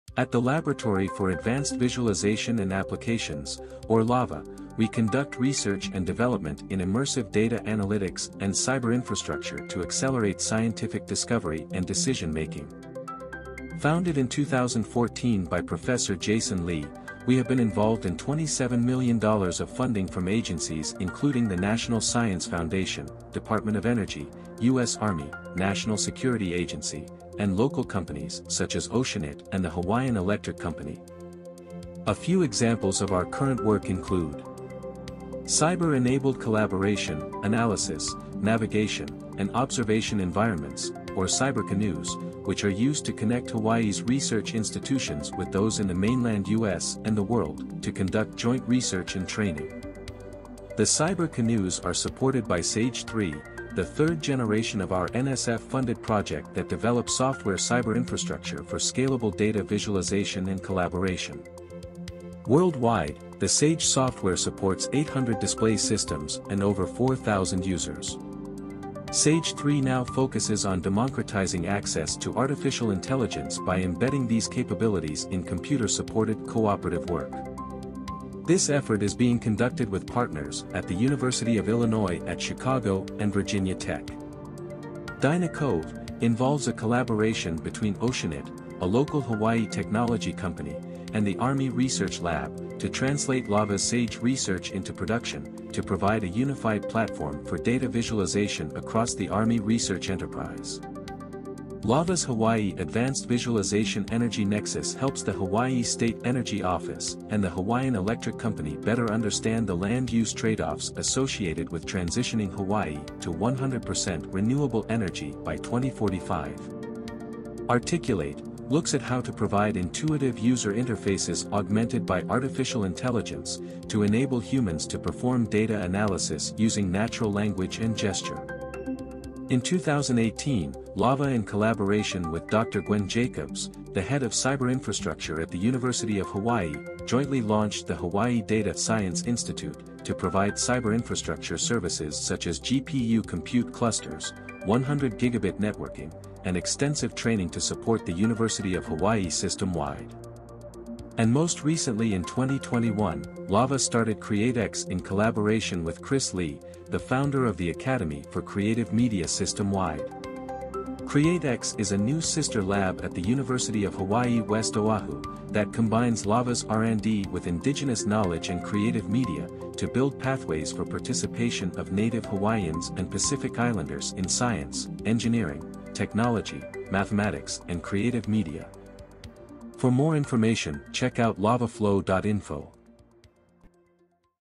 7 years of LAVA
Update on the Laboratory for Advanced Visualization & Applications at the University of Hawaii at Manoa since its establishment in 2014.